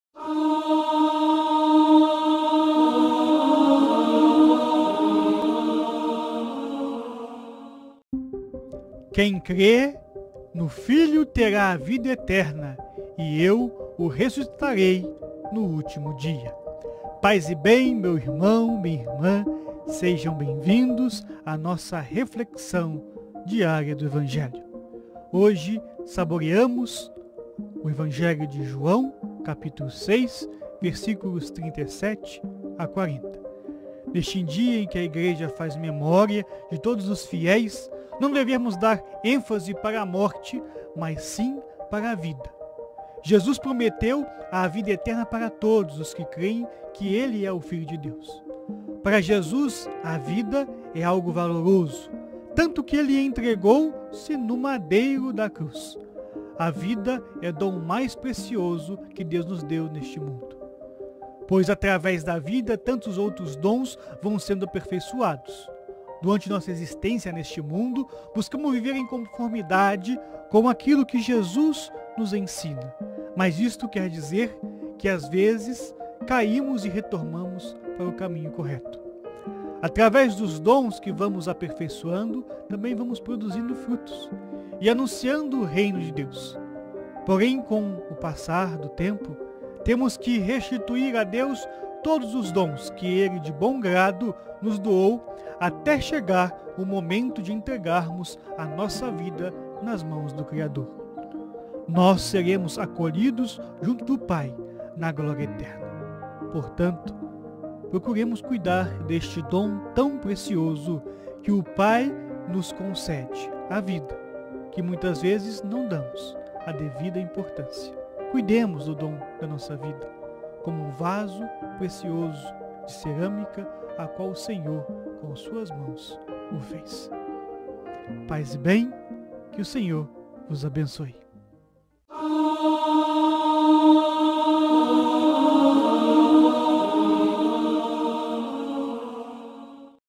Sabor da Palavra | Reflexão do Evangelho do dia 02/11/2020
Reflexão do Evangelho do dia 02/11/2020, com os Frades Franciscanos do Convento São Boaventura, de Campo Largo, no Paraná. O Evangelho deste domingo está em Jo 6,37-40.